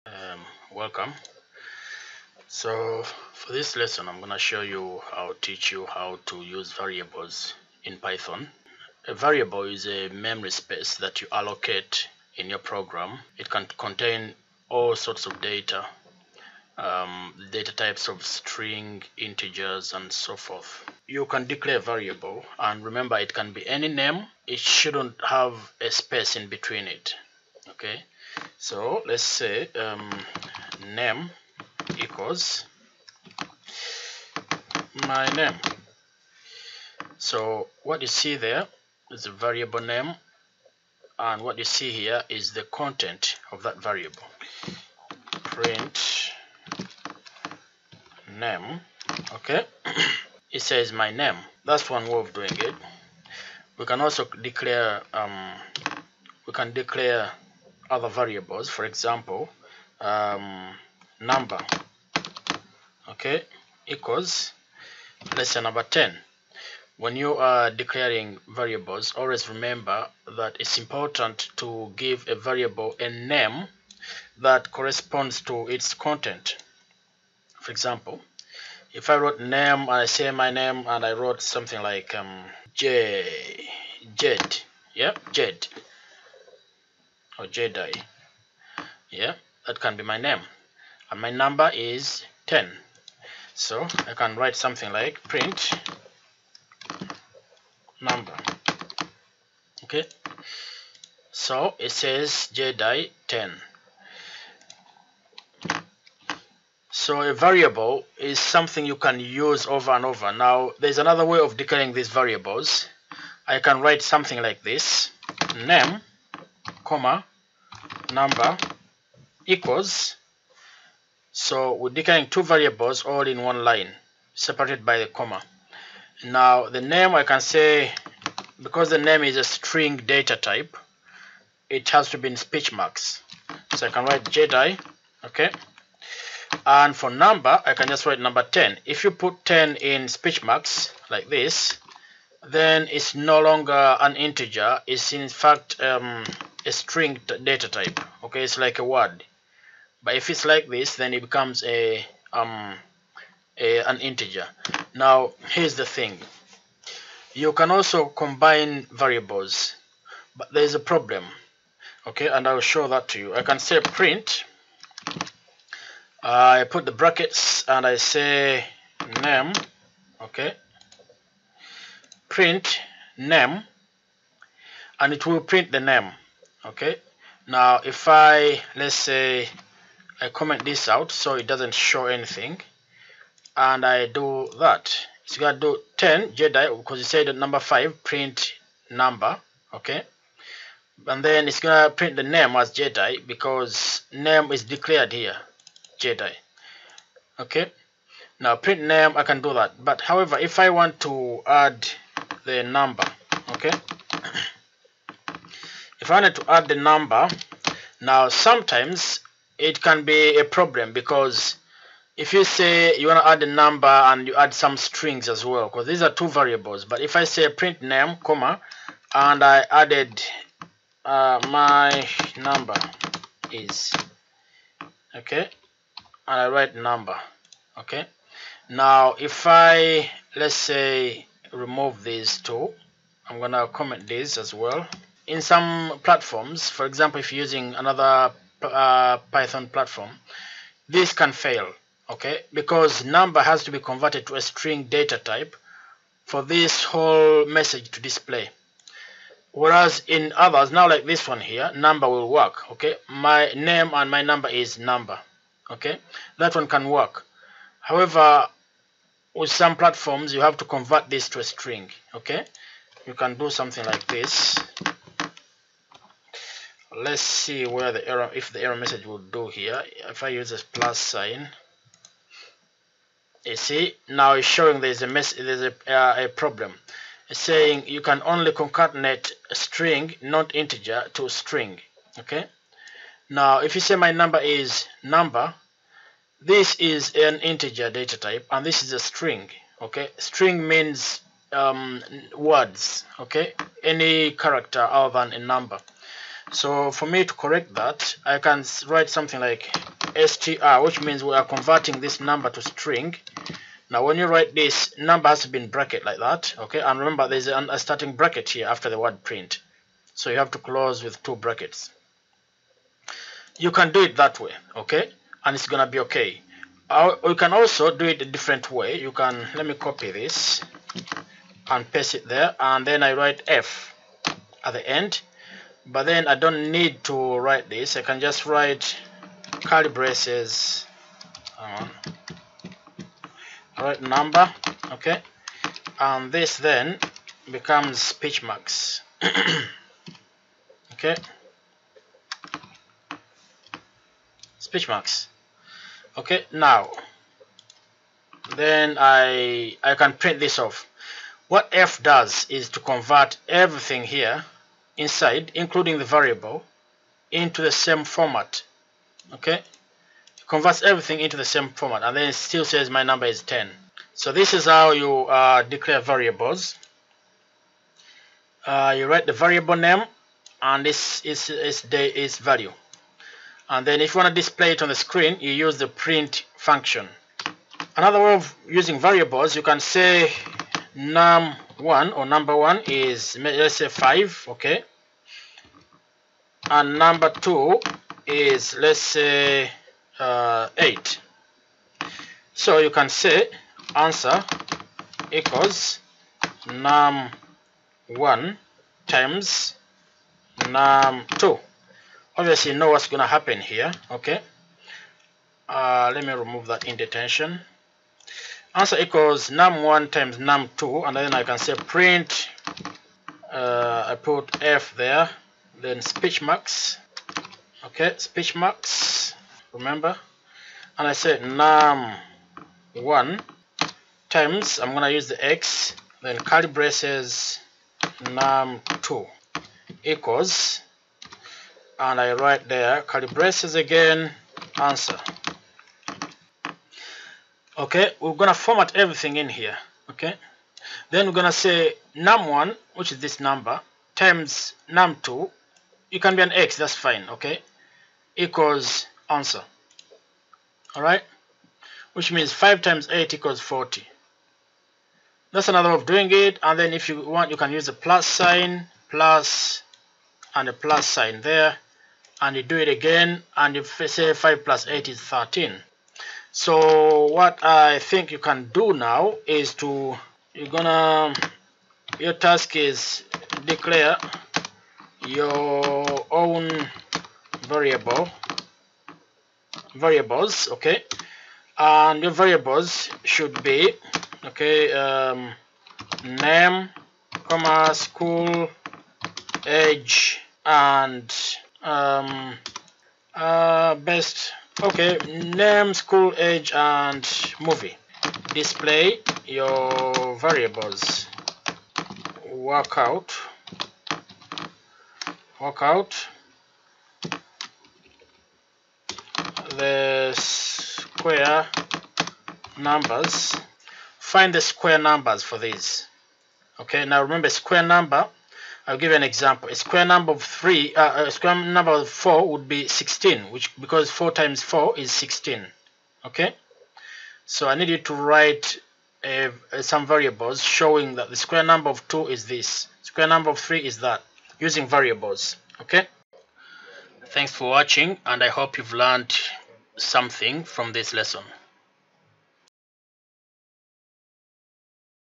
Using Variables in Python
For this lesson you will learn various ways of using variables in Python Programming.